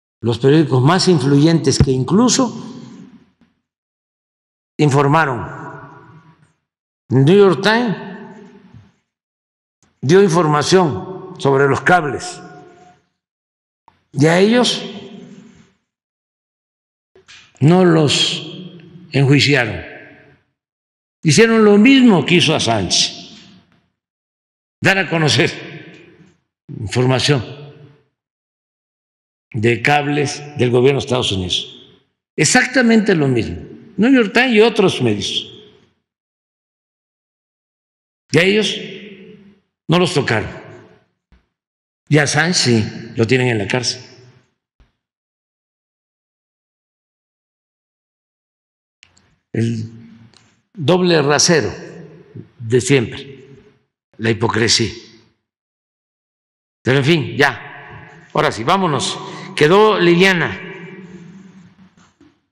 AMLO EXHIBE DOBLE RASERO DE NYT Y OTROS MEDIOS QUE HICIERON LO MISMO QUE ASSANGE, PERO ÉL ESTÁ PRESO
El presidente López Obrador volvió a exhibir al New York Times y otros medios de Estados Unidos al asegurar que a pesar de que han dado a conocer cables confidenciales, es decir, que hicieron lo mismo que el fundador de WikiLeaks Julian Assange, a ellos no lo tocaron y a Assange ahí lo tienen la cárcel, con el doble rasero de siempre.